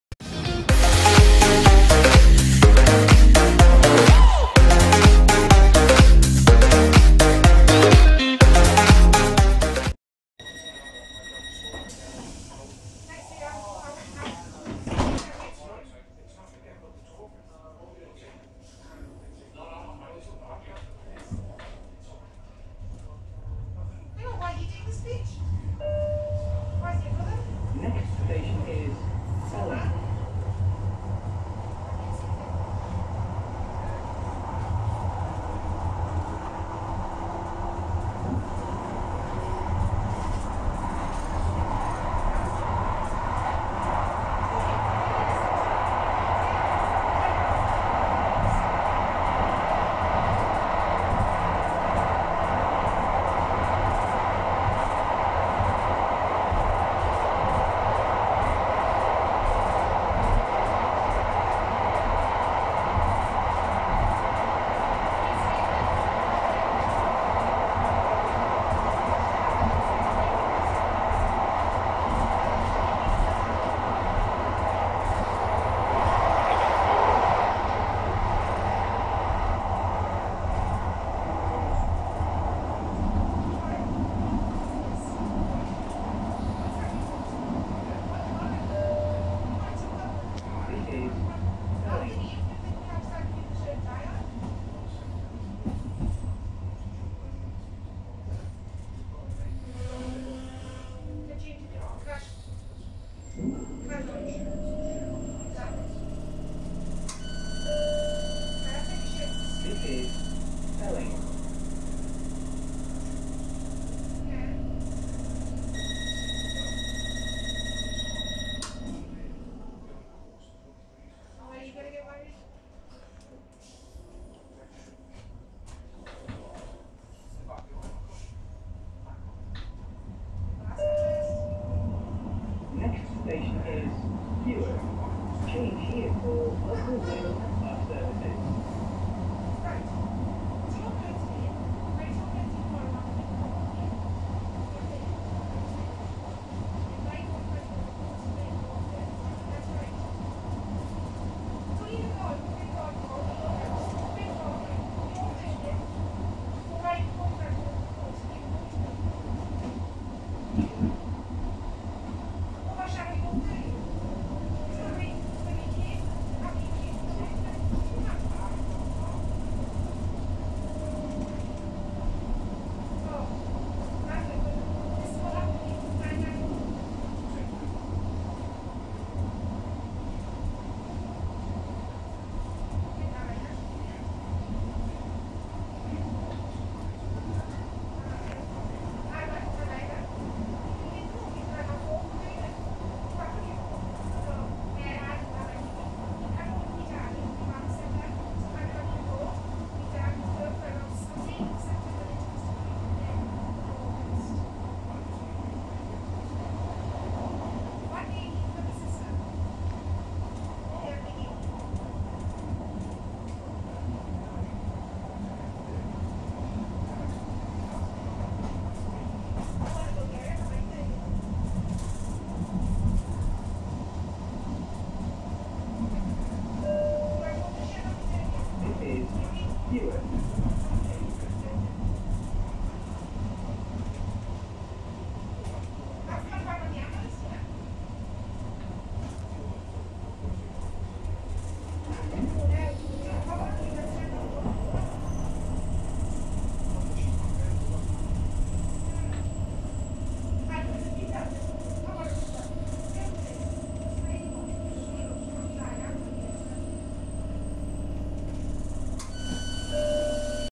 Tyne & Wear Metro|4040/4083|Class 599|Pelaw-151|Gateshead Stadium-Heworth.
Route:151

Class:599

Fleet code: 4040 & 4083

Livery: Tyne & Wear Metro: Yellow & Black

Location: Gateshead Stadium-Heworth